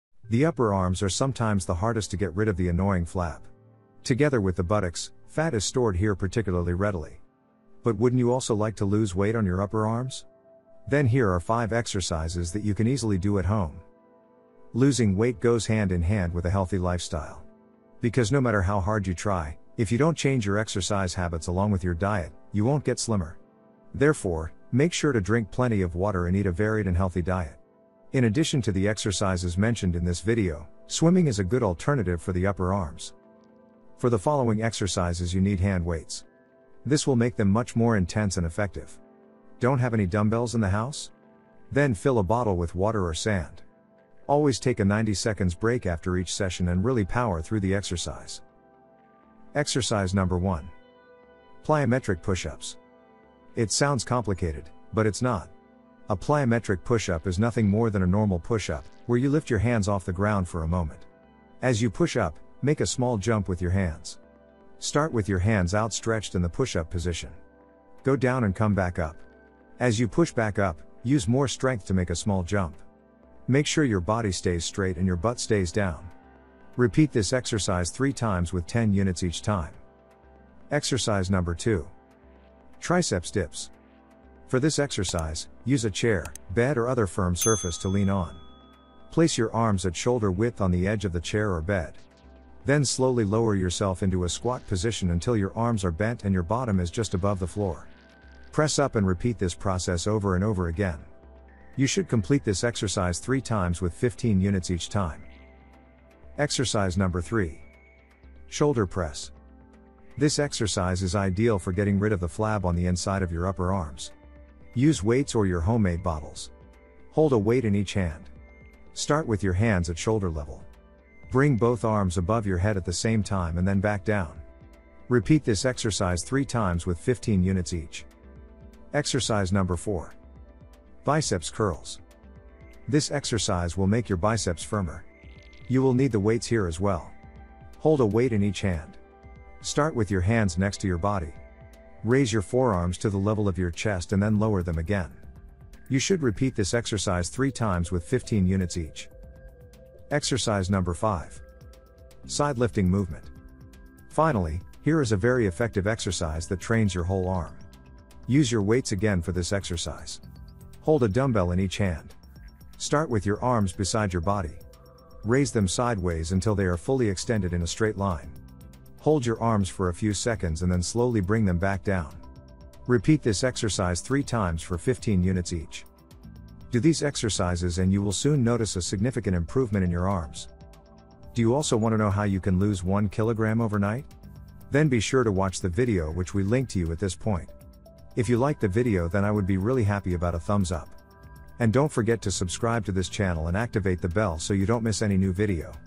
Do these 5 Short Exercises to get SLIMMER Upper Arms FAST 💥 (SUPER Easy) 🤯
Do you think your upper arms are too fat? - Here we show you how to get slim upper arms with 5 exercises!

Do you think your upper arms are too fat and would you like to lose some weight? Then this video is just right for you! Because here we show you 5 exercises with which you can lose weight on the upper arms. So your upper arms will be much slimmer and also the angle arms will be significantly less and you can lose some weight on the upper arms! Try it out but definitely once!